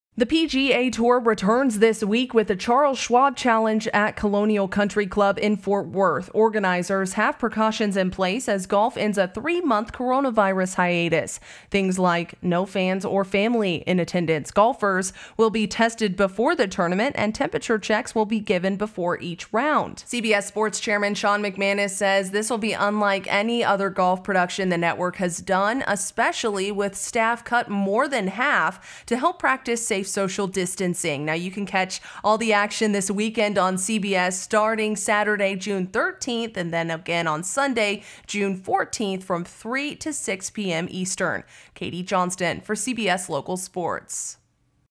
PGA Tour Returns With Charles Schwab Challenge
With The Charles Schwab Challenge, the PGA Tour offers the sports-starved public a taste of what live sports will look like in the coronavirus future. Katie Johnston reports.